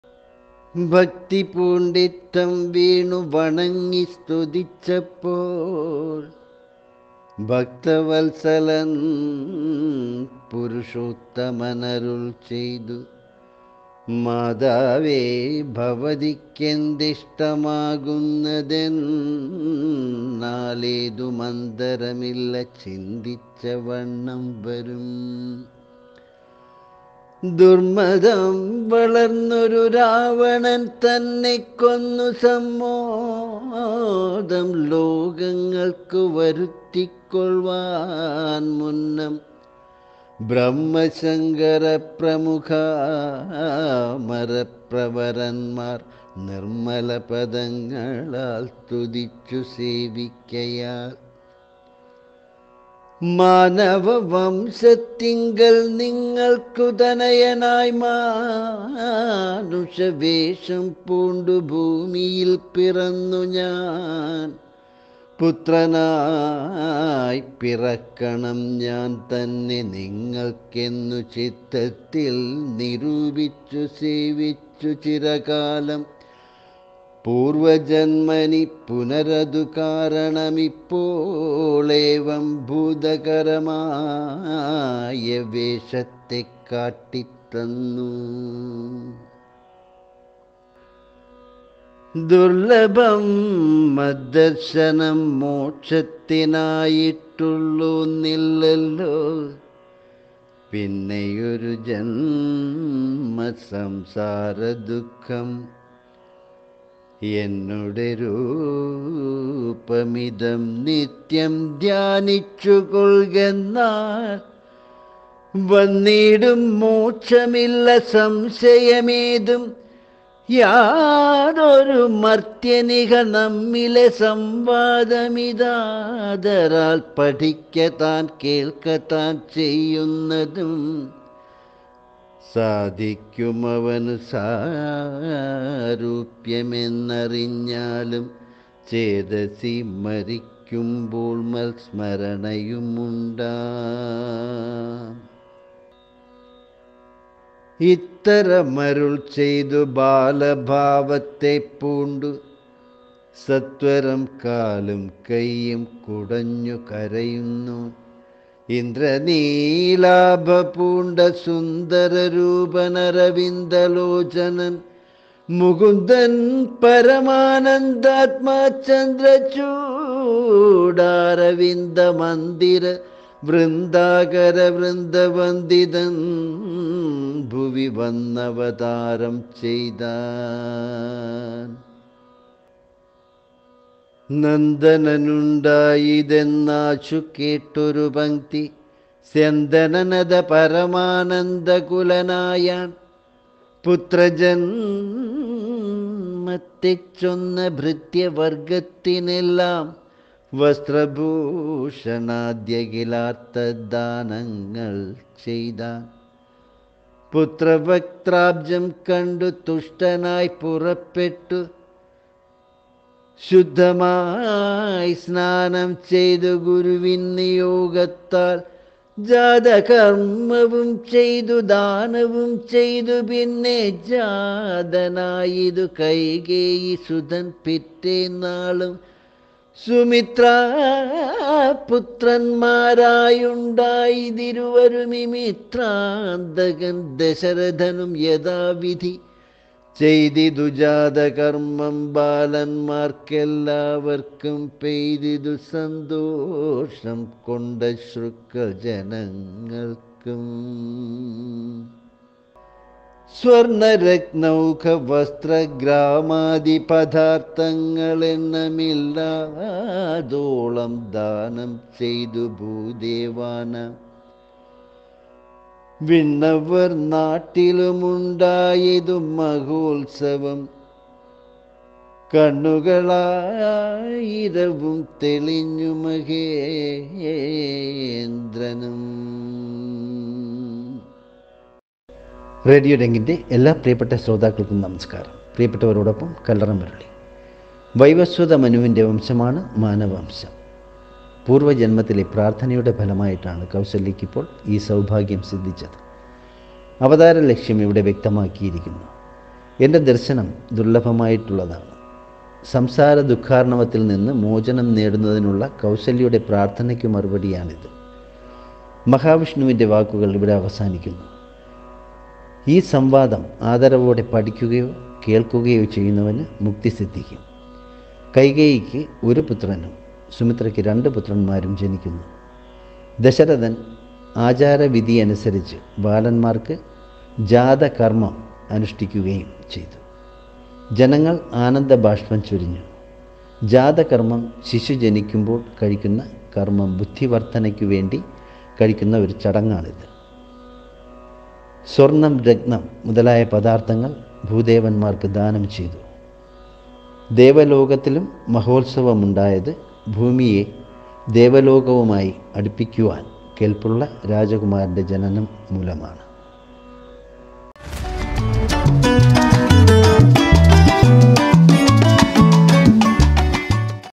കർക്കടകം 15 -ാം ദിനം രാമായണം പാരായണം RAMAYANAM PART 15 KALLARA MURALI
കർക്കടകം 15 -ാം ദിനം രാമായണം പാരായണം ചെയ്യേണ്ട ബാലകാണ്ഡം ഉമാ മഹേശ്വര സംവാദം ഭാഗം തുടർച്ച Adhyathma Ramayana Balakandam Part - 15 KALLARA MURALI